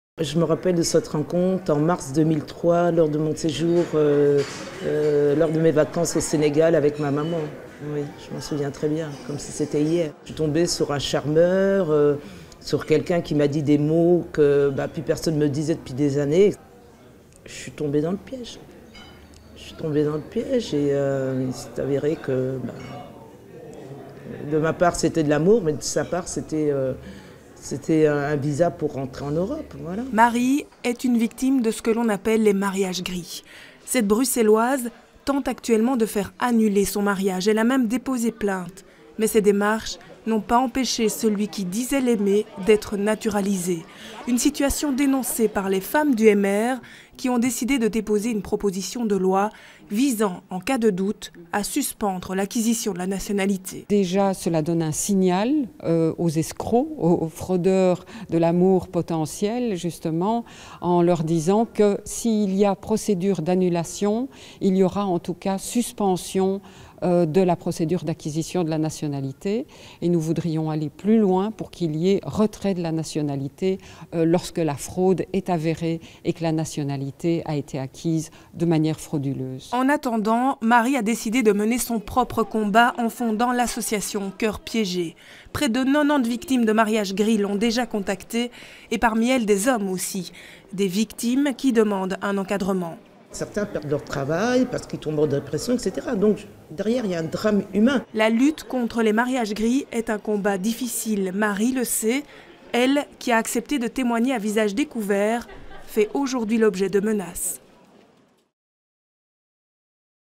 Lutte contre les "mariages gris" à Bruxelles
Les femmes MR de Bruxelles veulent lutter contre l'arnaque à l'amour : "le mariage gris" ou comment obtenir la nationalé belge en mentant sur ses sentiments.